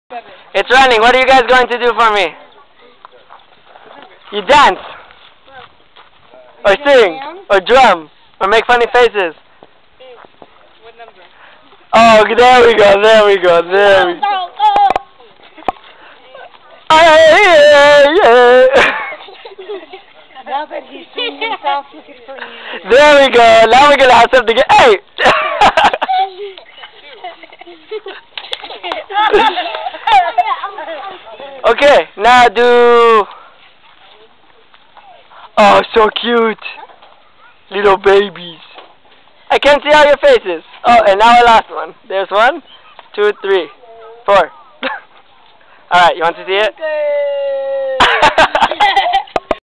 Children jumping in Kete Krachi's Village of Hope
Touch a Life's Village of Hope, based in Kete Krachi. First day after being rescued from children slavery for two of the boys. Don't mind the funny accent, just look at those smiles.